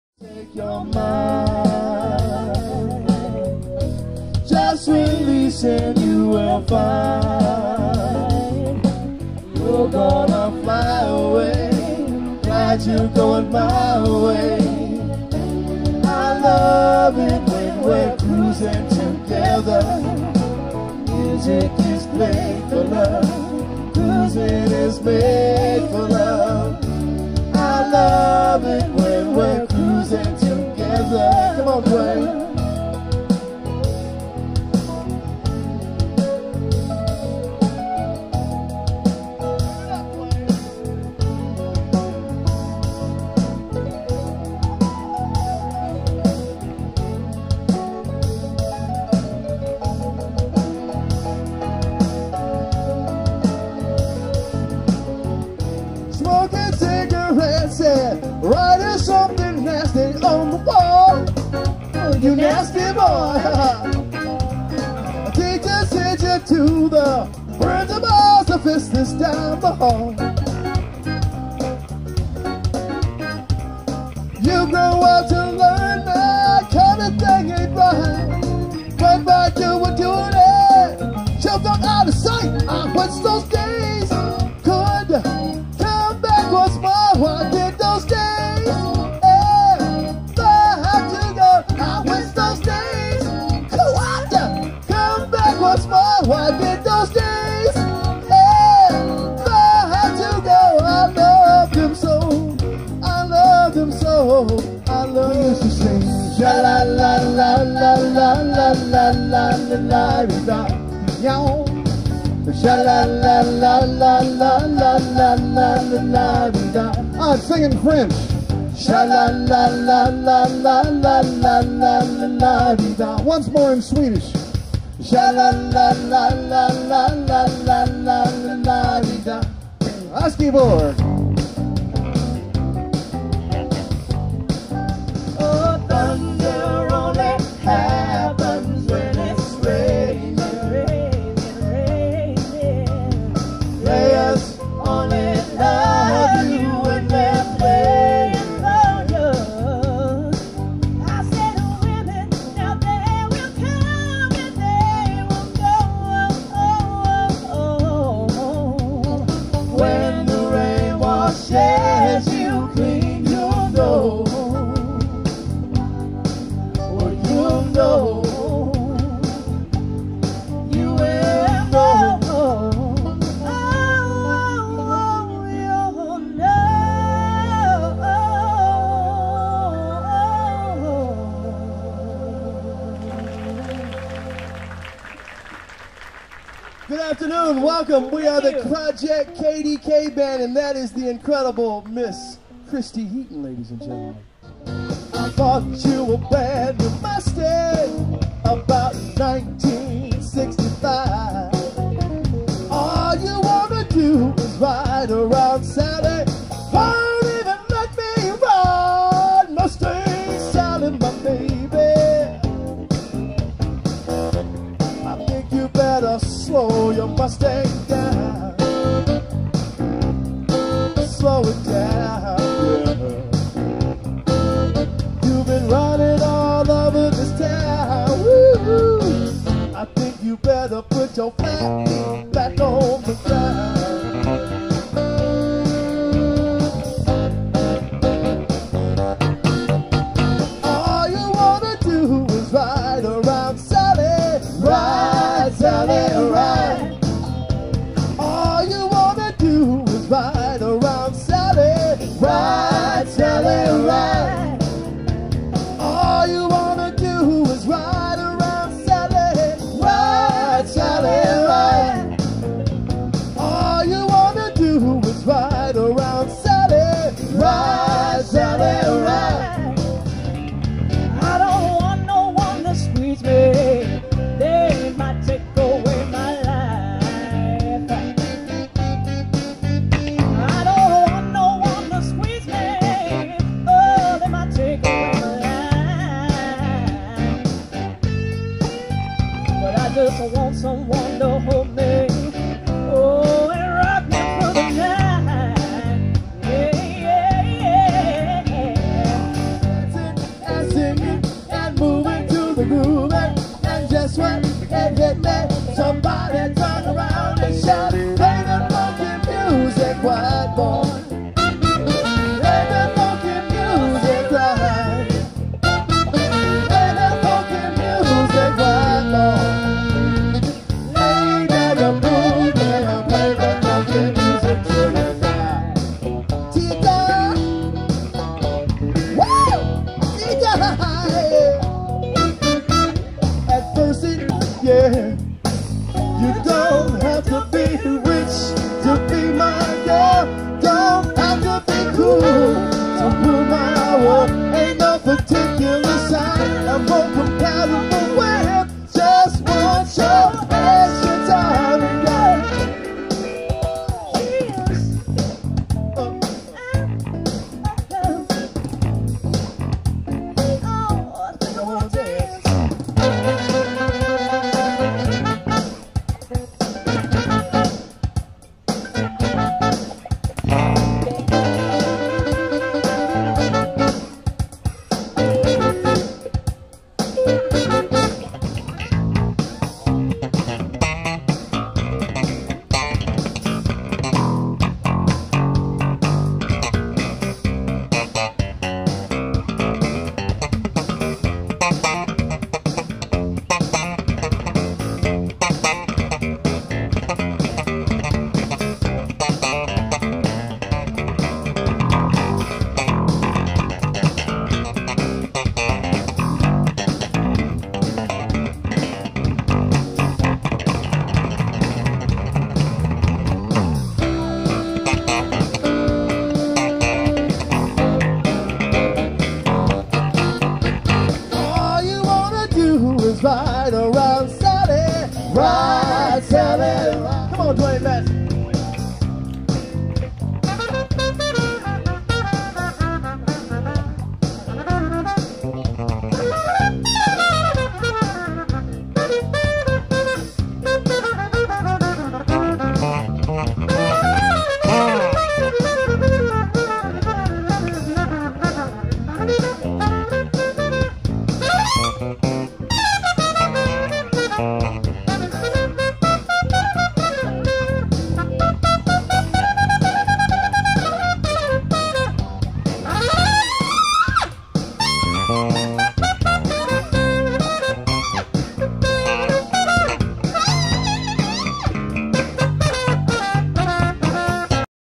Project KDK Band excerpts from live performance at Discovery Green
Short clips from live performance.